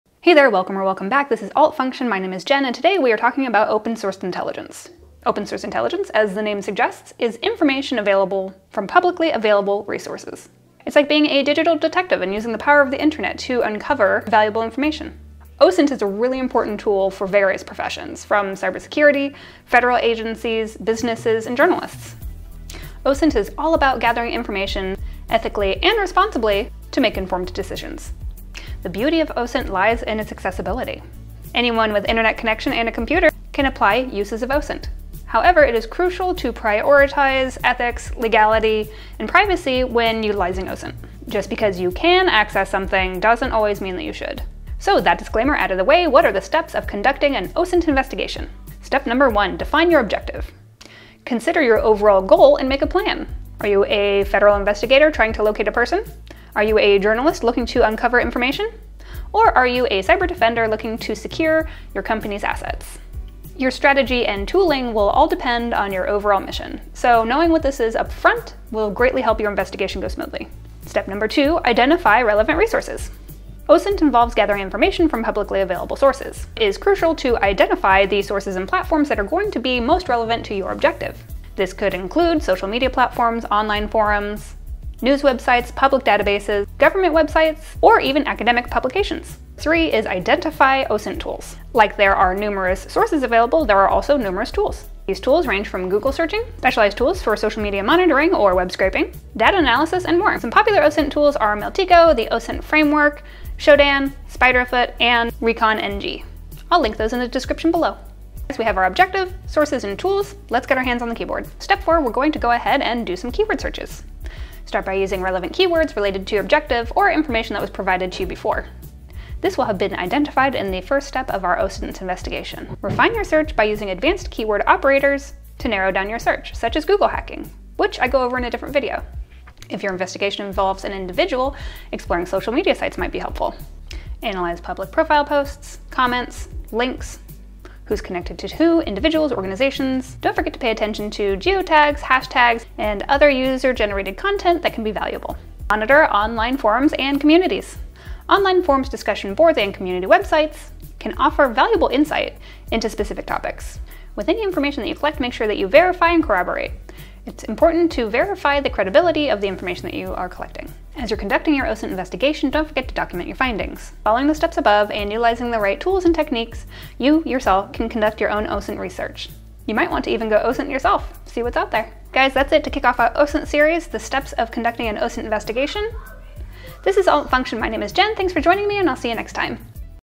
OSINT: How to Become an Intelligence Analyst
In this video is a high level kick starter to your OSINT investigation. I'll be covering some of the steps you will want to consider while developing an OSINT investigation. There are a ton of ways to go about it, my goal here is to get you thinking critically about your objectives when it comes to your target OSINT investigation. What information are you looking for? What tools and resources will you need? Break out your investigative cap and put hands on keyboard it's time to dive into OSINT!

I'd love to make videos for these specific tools in the future! :)

Mentioned Sources:
Advanced Google Searching
Forums
Online Communities
Government Documentation
Research Papers

OSINT Tools:
Google "Hacking" aka Google Dorks